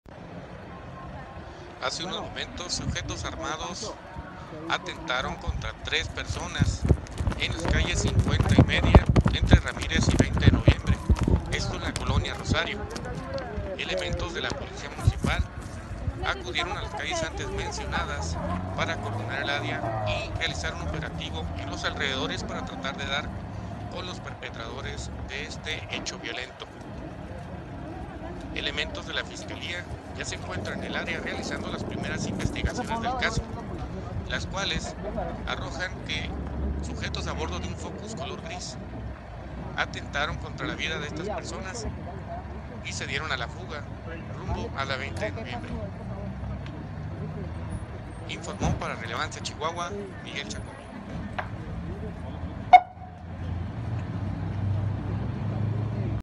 Ejecutan a tres personas en la Rosario
Chihuahua.- Hace unos momentos sujetos armados atentaron  en contra de tres personas que se encontraban abordo de un vehículo en la colonia Rosario.